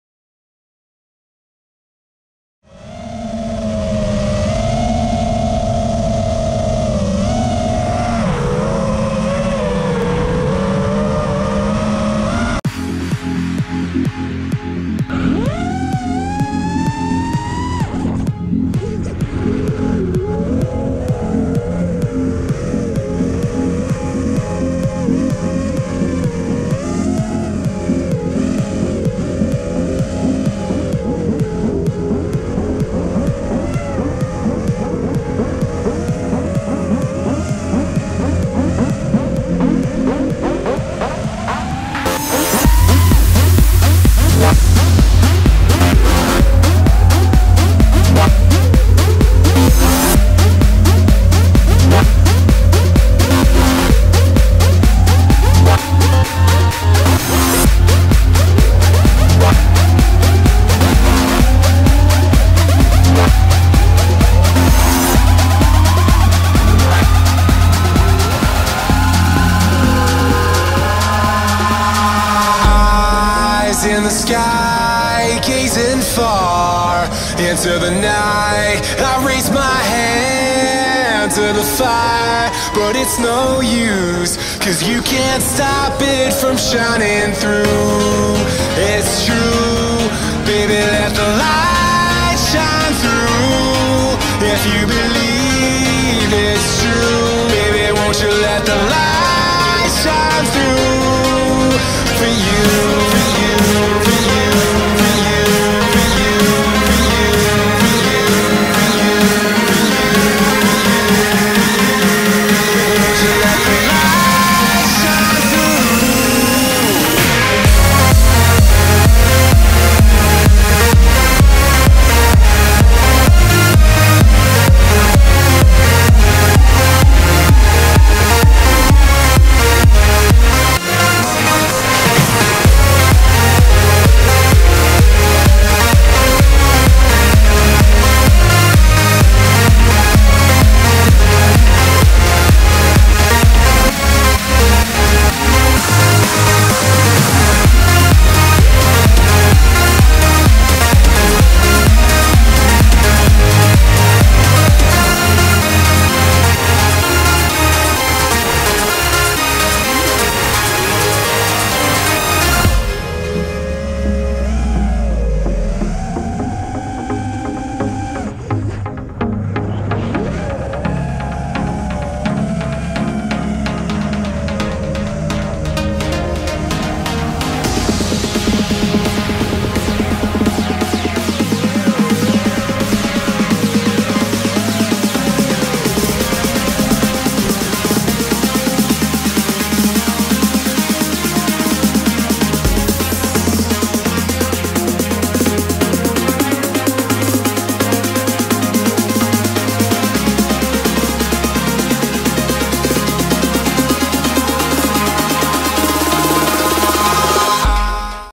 Taking the Nubb Full Pack Dare Again
A full pack, no edits just as Nubb dared us all to do a while back.  Sunday afternoon cruising.

Oh yeah, I was just trying out the DALPROP Cyclone 5040s, 3rd pack.